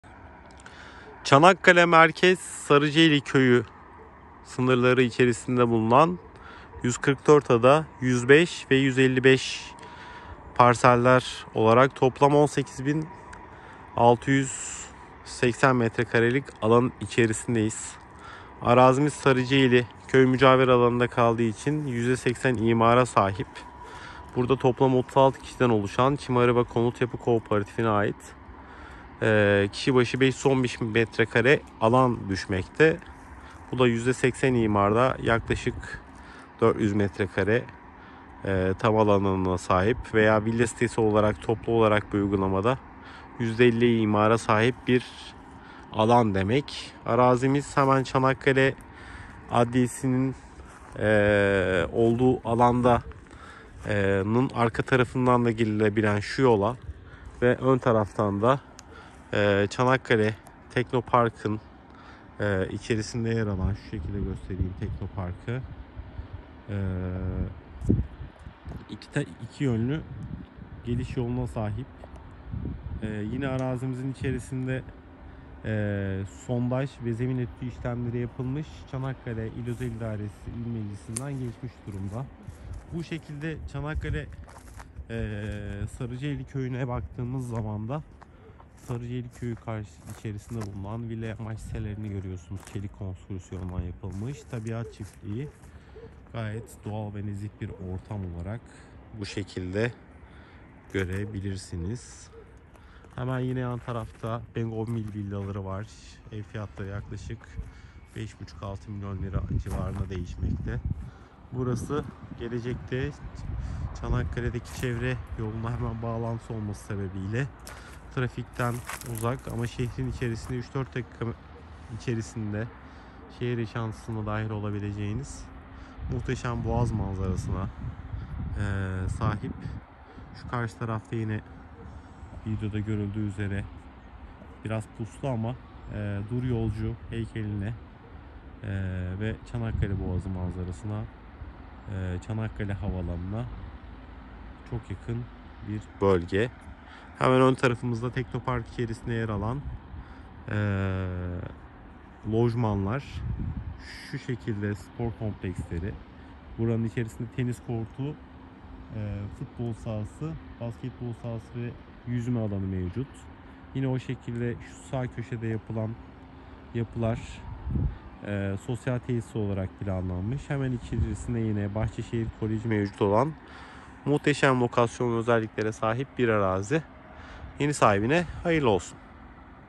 ÇANAKKALE MERKEZ BOĞAZ MANZARALI İMARLI SATILIK ARSA
Çanakkale Merkez Sarıcaeli Köyü toplam 18.568 metrekarelik köy imarlı arazide bulunan 515’lik arsa satılıktır. Arsamız; Çanakkale Boğazı ve doğa manzaralı, önü kapanmaz yapıda ve tüm evlerin denizi görebileceği şekilde arazi yapısına sahiptir. Çanakkale Teknopark’a 300 metre, Bahçeşehir Kolejine 500 metre, Çanakkale çevre yoluna 900 metre, Çanakkale Adliyesine 1.5 kilometre, Çanakkale 18 Mart Üniversitesi ile Hastanesine 2.5 kilometre, Esas 17 Burda AVM’ye 2.5 kilometre, Çanakkale Havaalanına 3 kilometre ve Çanakkale Merkez ile Feribot İskelesine 5 kilometre mesafededir.

Araziye hem adliye yönünden hem de köy içinden ulaşım mevcuttur. 36 hissedarlı arsamızda hisse başına yüzden seksen imarlı 515 metrekare alan düşmekte olup villa sitesi yapılması planlanmaktadır.

Ayrıntılı bilgi için bize ulaşarak arazimizi yerinde görmek üzere randevu oluşturabilirsiniz.

ALANKA GAYRİMENKUL